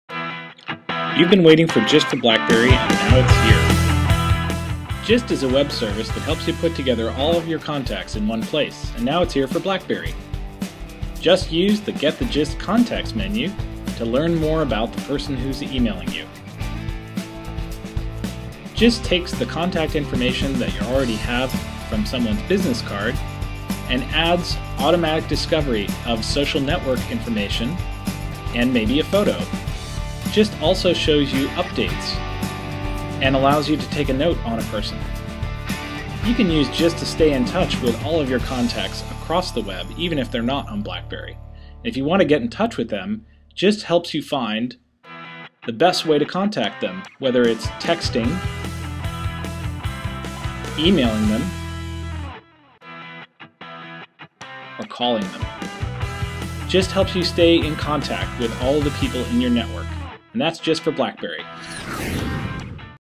Gist for BlackBerry - Get the Gist
Gist is a web service that gives you all your contacts in one place updated with the latest news everywhere you need it. You can use Gist on the web, in email like Gmail or Outlook, or on your mobile.

This brief video demonstrates how to use Gist on your BlackBerry Torch. You can learn more about Gist for BlackBerry smartphones